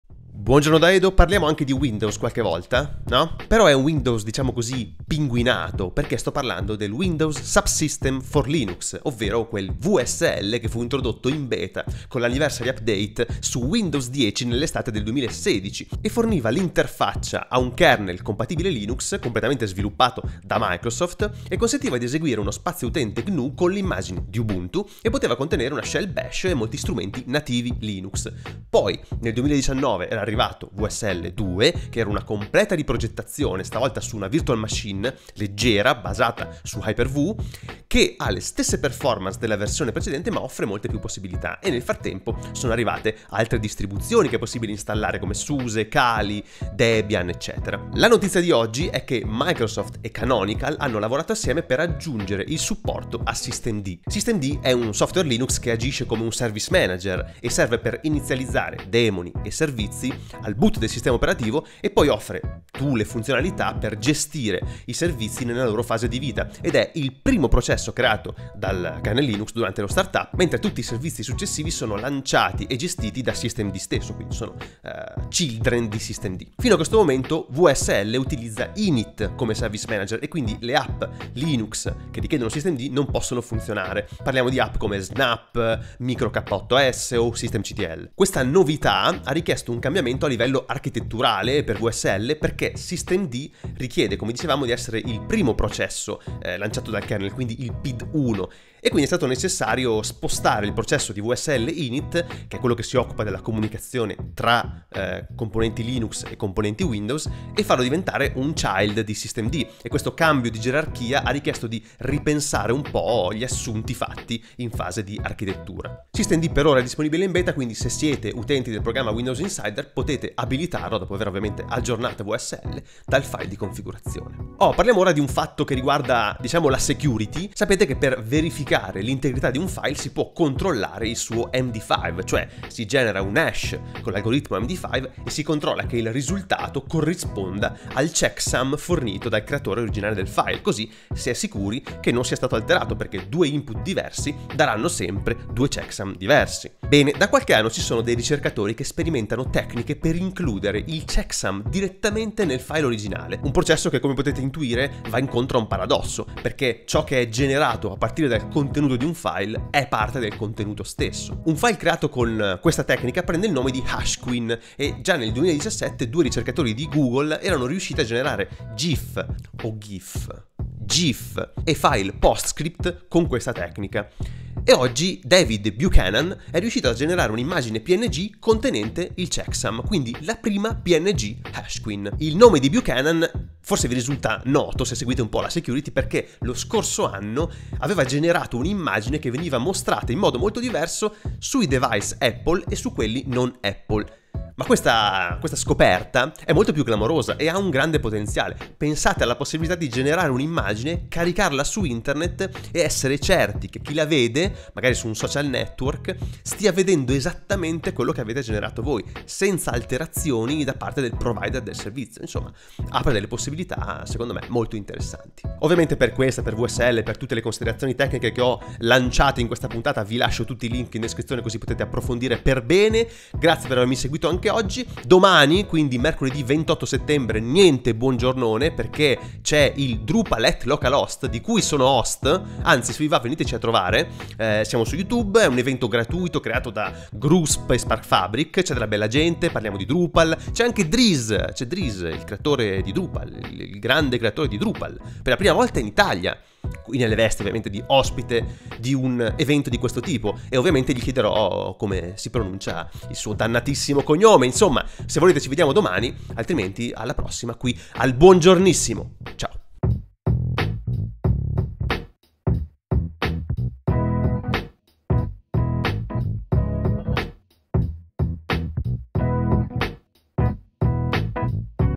systemd su WSL e PNG che contengono il loro stesso MD5
/** Links */

This image shows its own MD5 checksum — and it's kind of a big deal